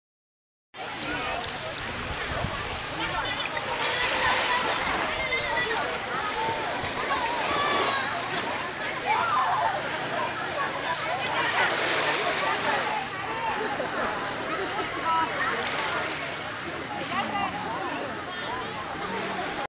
Starfish @ Koh Kradan, Krabi, Thailand 2012
This was my first time I saw a starfish =) .. it was blue, grey & orange coloured